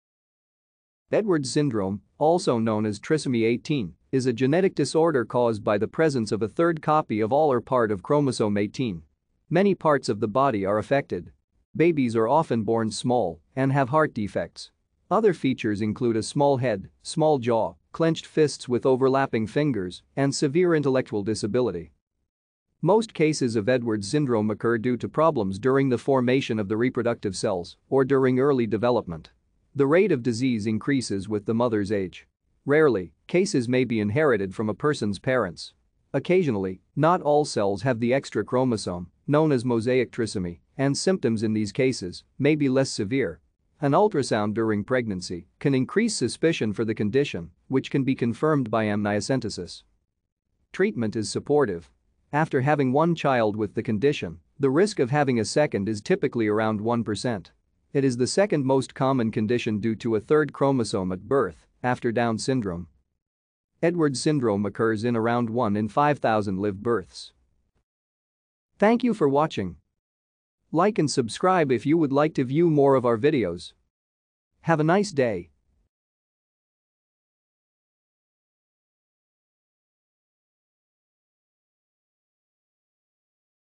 Edwards Syndrome
Edwards syndrome, also known as trisomy 18, is a genetic disorder caused by the presence of a third copy of all or part of chromosome 18. Many parts of the body are affected. Babies are often born small and have heart defects. Other features include a small head, small jaw, clenched fists with overlapping fingers, and severe intellectual disability.